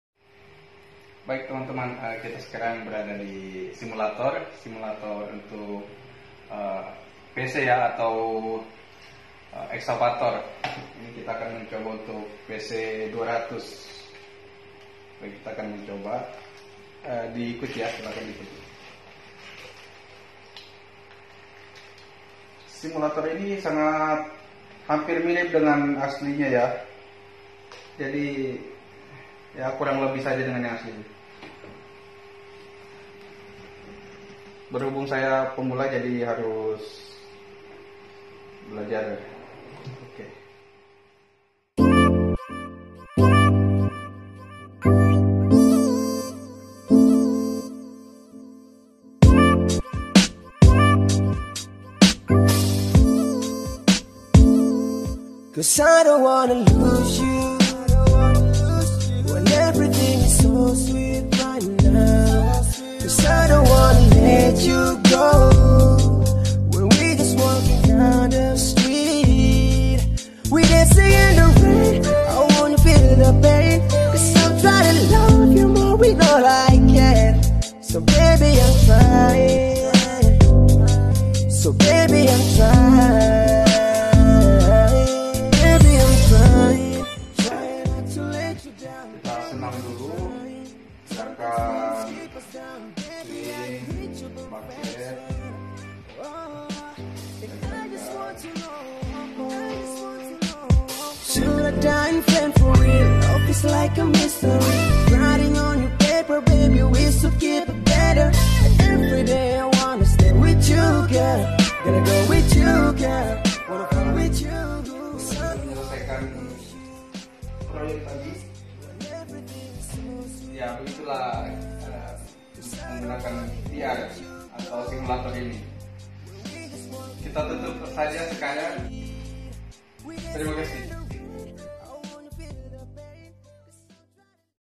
Simulator PC 200 IMMERSIVE PT.BUMA SITE KIDECO
Dalam kesempatan ini saya mencoba menjajal teknologi mutakhir simulator alat berat yang ada di dunia pertambangan yaitu  IMMERSIVE TECHNOGIS dengan unit KOMATSU PC 200.

Suatu pengalaman baru Bagi saya mencoba teknologi ini dan sangat menyenangkan. Semoga video ini bermanfaat untuk teman-teman semua.